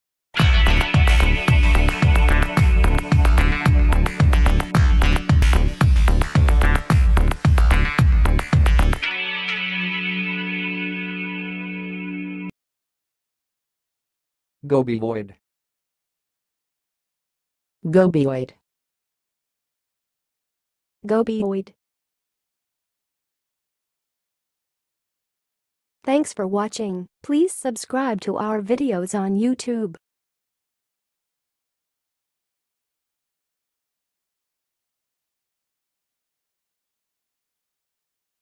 GOBIOID - HOW TO PRONOUNCE IT?
GOBIOID - Unlocking the correct sound of Gobioid in 2023

How do you pronounce 'gobioid'?
What is the stress pattern in the word 'gobioid'?
Can you provide an alternative pronunciation for 'gobioid'?
GOBIOID Meaning And Definition:
Gobioid refers to a category or classification of fish species belonging to the family Gobiidae.
Gobioids are predominantly small, bottom-dwelling fish that are found in marine, brackish, and freshwater habitats around the world.
They are known for their unique body shapes, which are often elongated and cylindrical, and their ability to adapt to various environments.
Gobioids are highly diverse in terms of their appearance, behavior, and ecological roles, making them a fascinating group of fish to study.
GOBIOID Pronunciation:
To pronounce 'gobioid', you can break it down into syllables as go-BY-oid.
The stress is on the second syllable.
Alternatively, you can pronounce it as goh-BEE-oid, with the stress on the first syllable.
Use GOBIOID In Your Speech:
1. The gobioid fish is well-camouflaged among the rocks.
2. Gobioids are commonly found in estuaries, where freshwater meets the sea.
3. Researchers are studying the adaptive features of gobioids in different environments
Please answer in comments for the most popular GOBIOID questions:
1. What is the family name of gobioids?
2. Where can you find gobioids?
3. How do gobioids adapt to diverse habitats?
4. What are some characteristics of gobioid fish morphology?
5. What are the ecological roles of gobioids in their habitats?